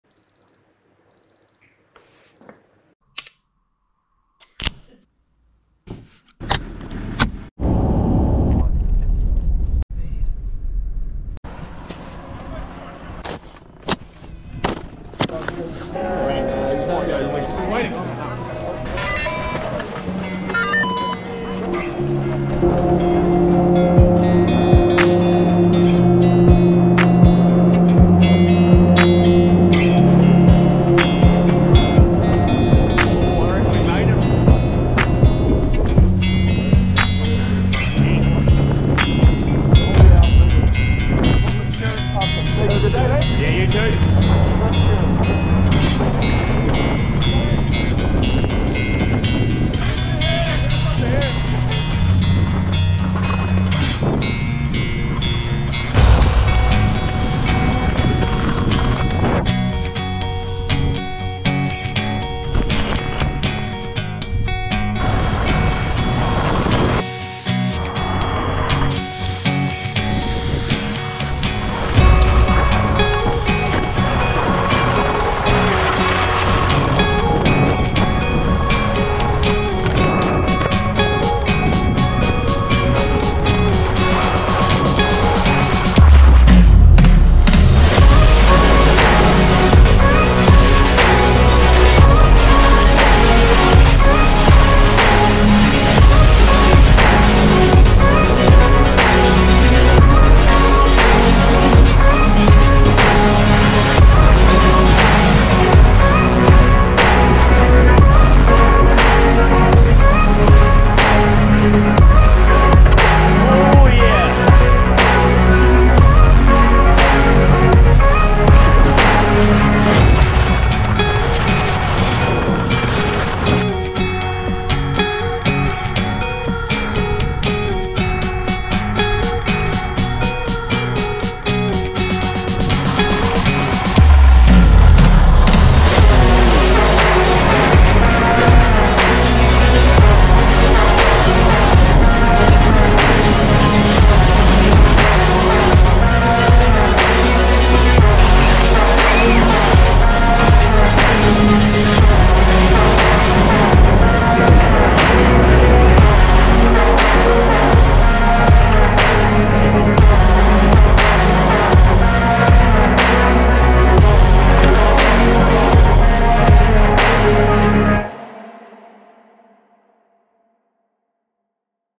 MT HUTT Opening Day 2023
We are back the 2023 Kiwi ski season is on. Come with join us at Mt Hutt's 2023 opening day.  New Snow, New Skis and a hole lot of anticipation winter here we come!!